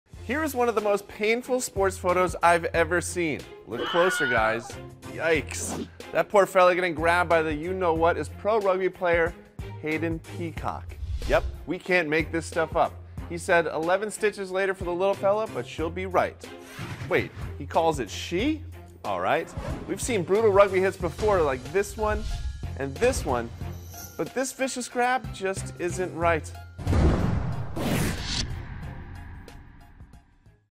This photo of a rugby player being grabbed by the groin will make you feel lightheaded
A pro rugby player needed 11 stitches after this nasty takedown. Ouch.